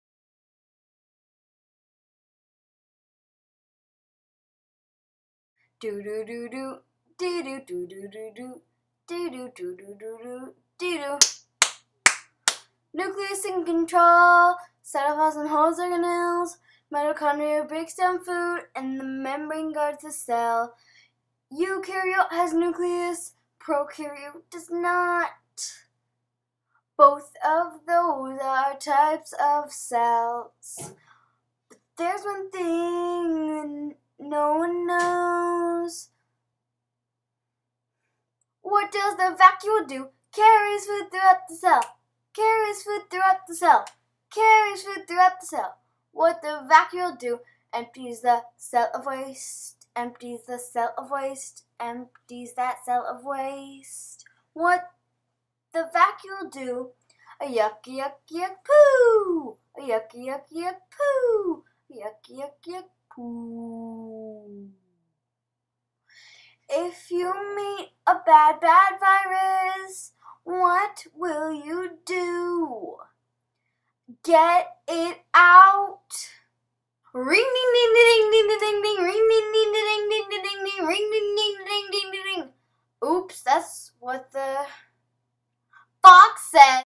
What does the Vacuole do?
A science video with claymation that will help students learn the jobs of organelles in a cell. The video contains lyrics to a song by Carleigh Templin